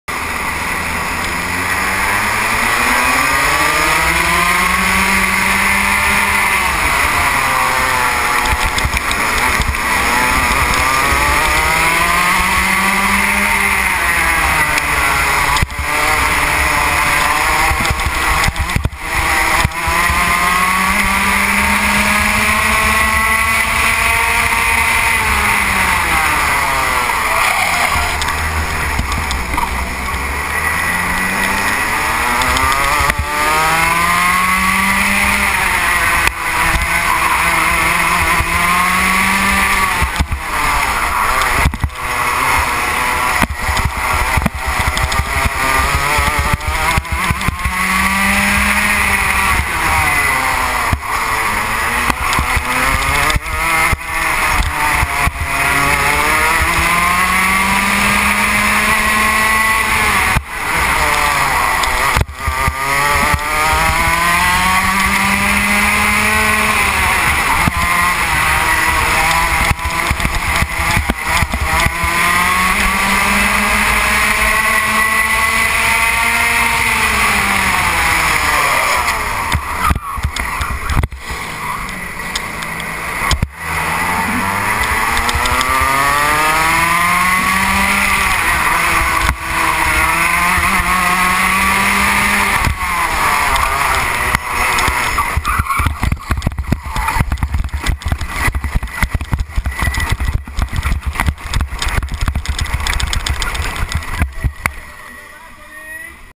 Gokart OB Győr Eto Park 2012 Rotax Max Masters 2. futam Nyitray Béla LGK
Kicsit rövidre sikerült, de kivédhetetlen volt a baleset.